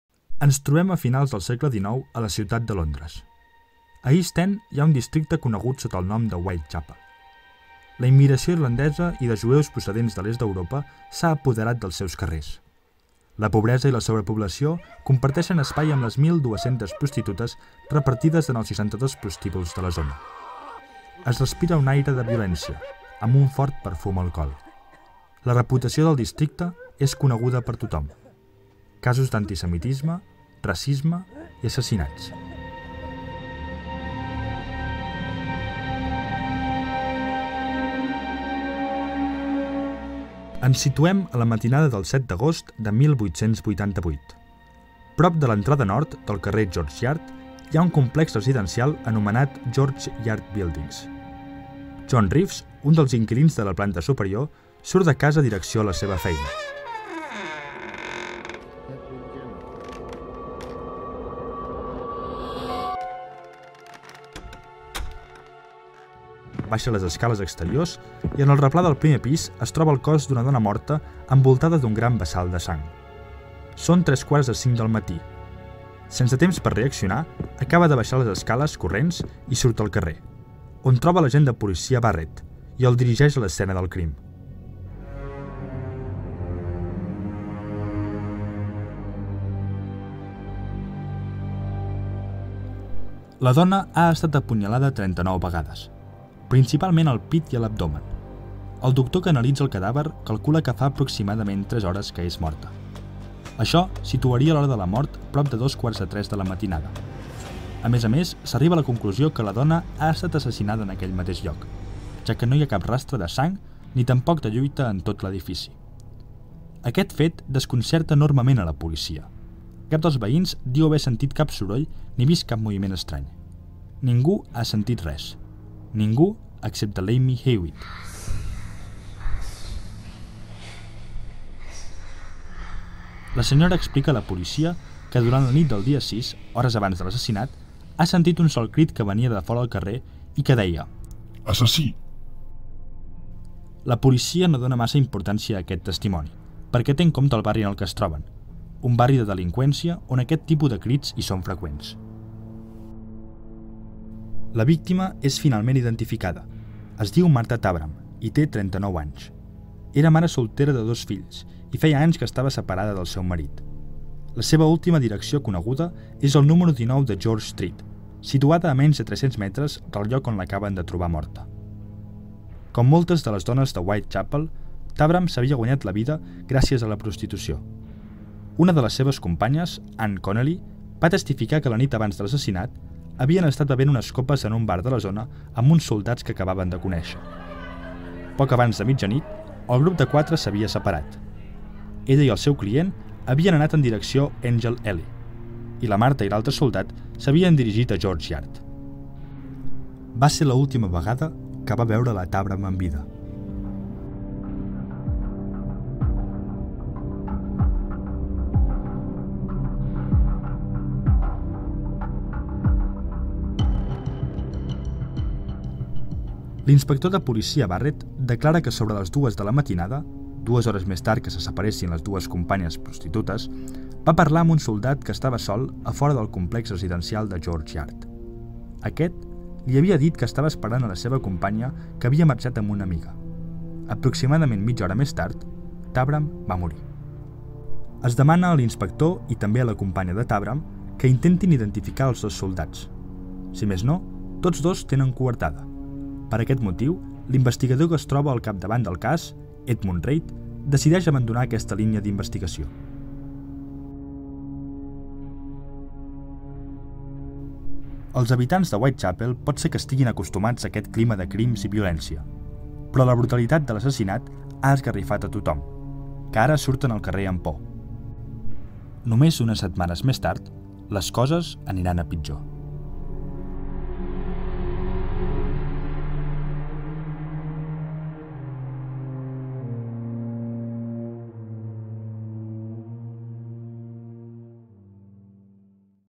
Episodi 1: Martha Tabram - Podcast Jack l'Esbudellador
Primer episodi del Podcast sobre Jack l'Esbudellador (Jack the Ripper). El 7 d'agost de 1888, a Londres, un home surt de casa per anar a treballar i es troba amb el cadàver d'una dona apunyalada 39 vegades. És el primer d'una sèrie de crims terribles que deixaran Londres completament desconcertada i atemorida.